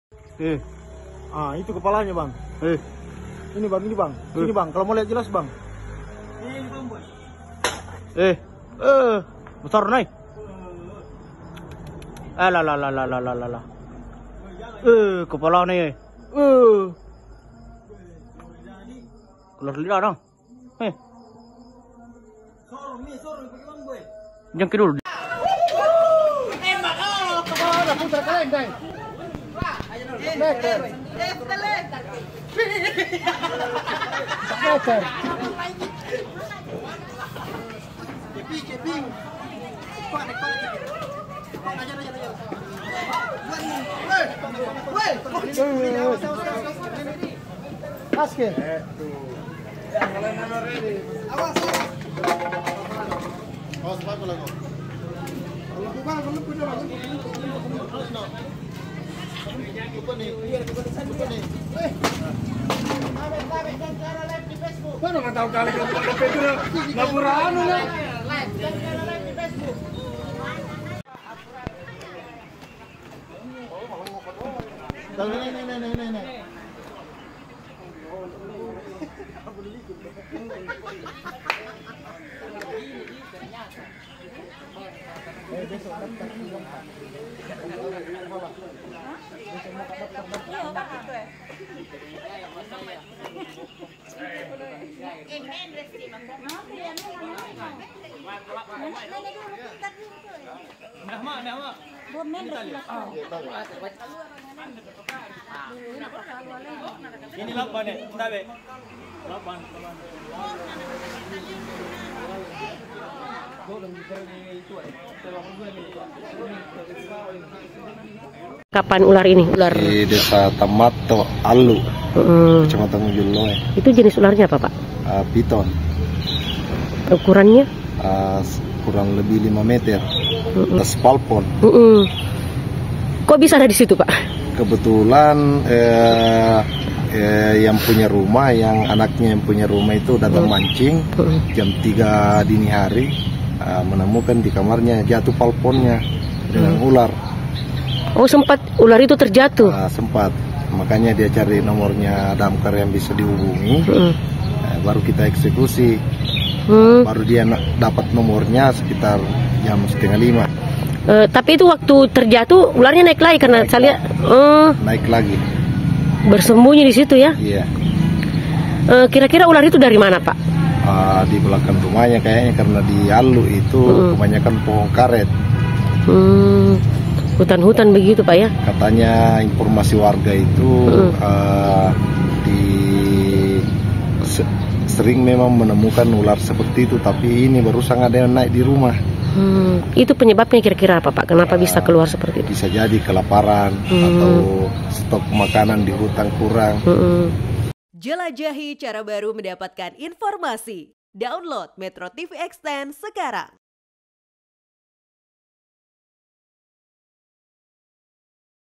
Petugas Damkar Bantu Evakuasi Ular Piton Berukuran Besar di Plafon Rumah
MetroTV, seekor ular pyton ditangkap warga di Dusun Allu Kecamatan Ujung Loe. Ular pyton sepanjang kurang lebih 5 meter ini ditemukan melilit diatas plafon rumah warga. Petugas damkar yang mendatangi lokasi langsung melakukan evakuasi dengan alat seadanya.